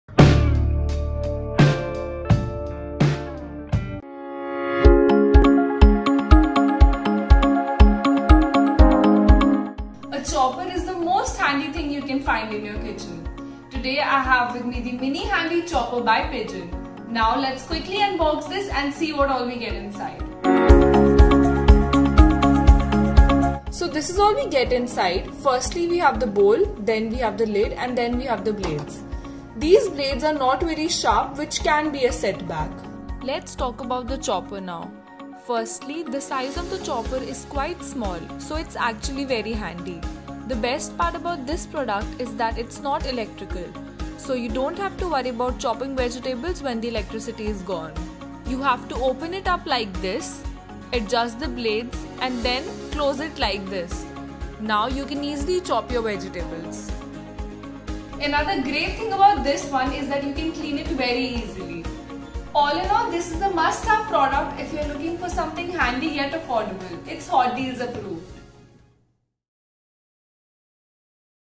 Papa Kitchen All Time Best Product Reviews (Handy Mini Plastic Chopper)
Hi Friends,
This video is on Pigeon chopper - by Stovekraft New Handy Mini Plastic Chopper with 3 Blades. I have shared the demo of the product and also done the review for it.